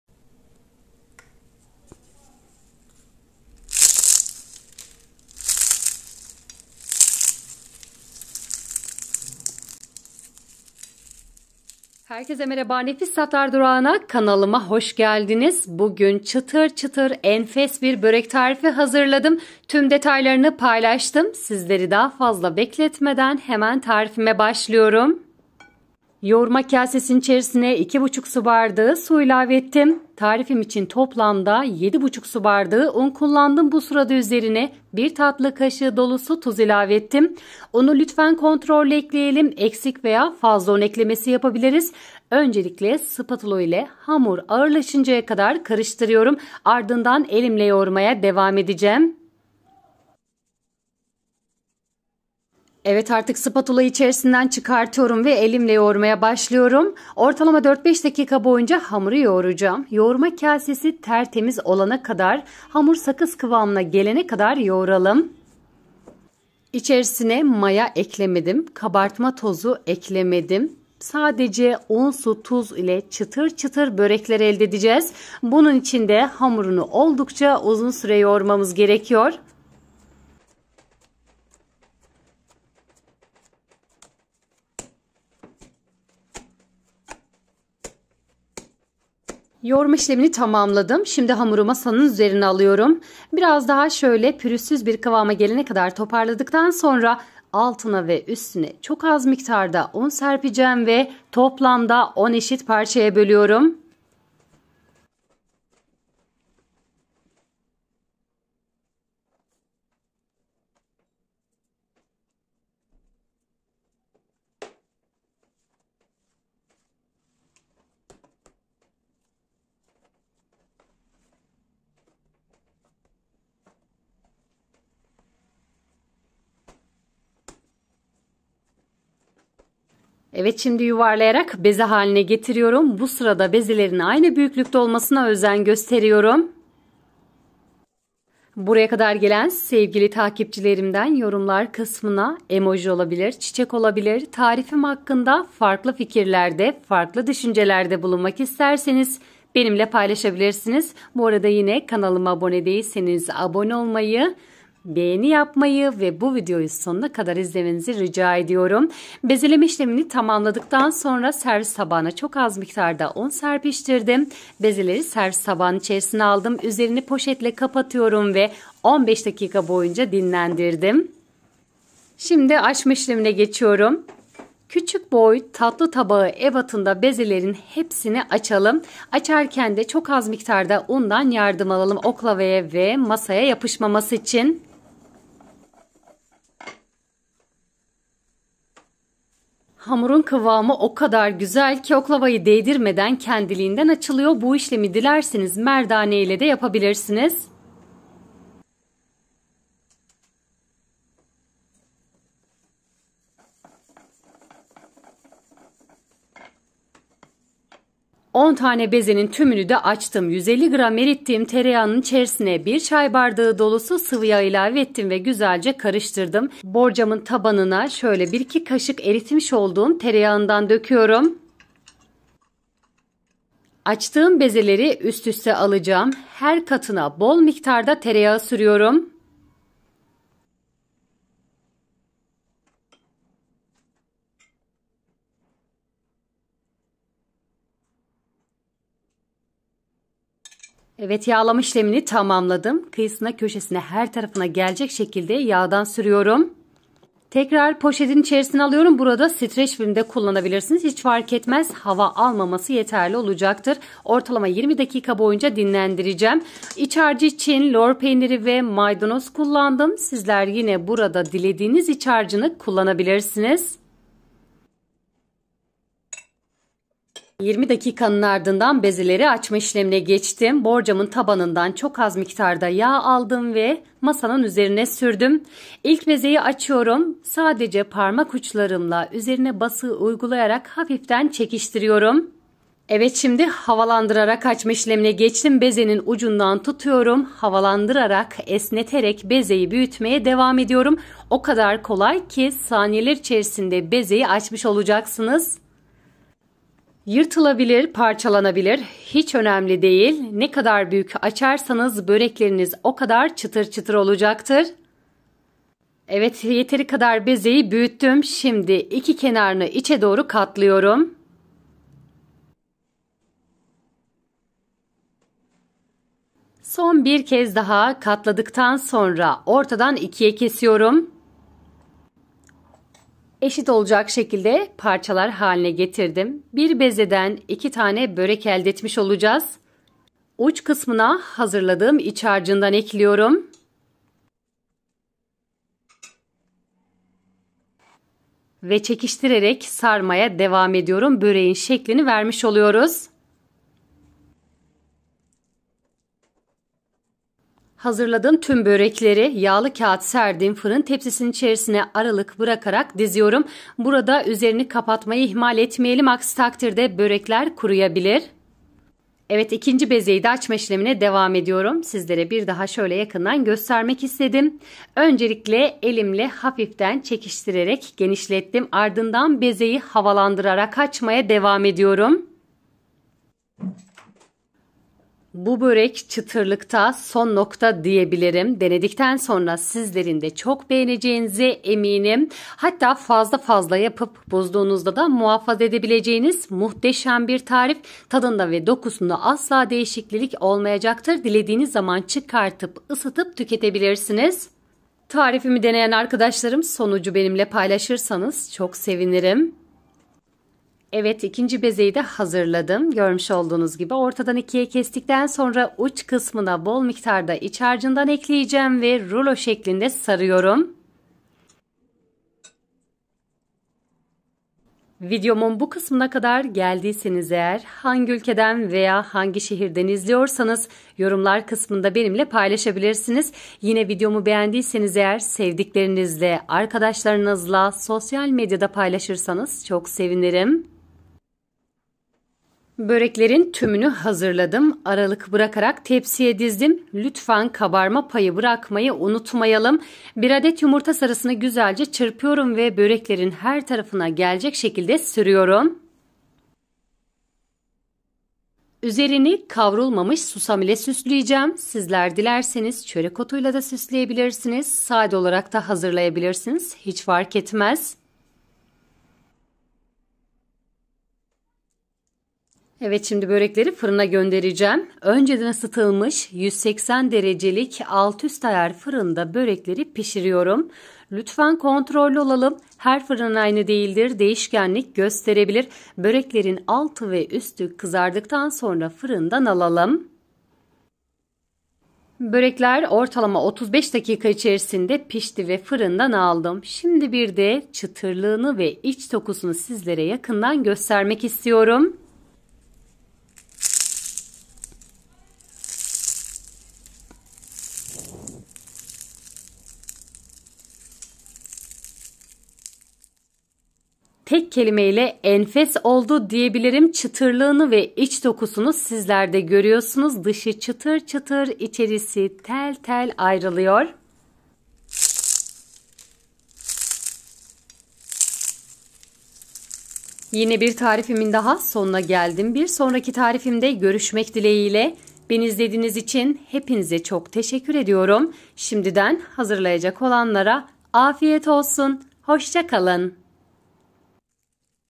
O Kadar KOLAY Kİ Neden Daha Önceden Yapmadım🤔Sadece Un,Su,Tuz ile‼️ ÇITIR Çıtır BÖREK TARİFİ 💯
Malzemeler;
2.5 bardak su
1 tatlı kaşığı tuz
6.5/7 su bardağı un

arasına sürmek için;
150 gr tereyağı veya margarin
1 çay bardağı sıvıyağ
Peynir
Maydanoz

üzerine;
yumurta sarısı
susam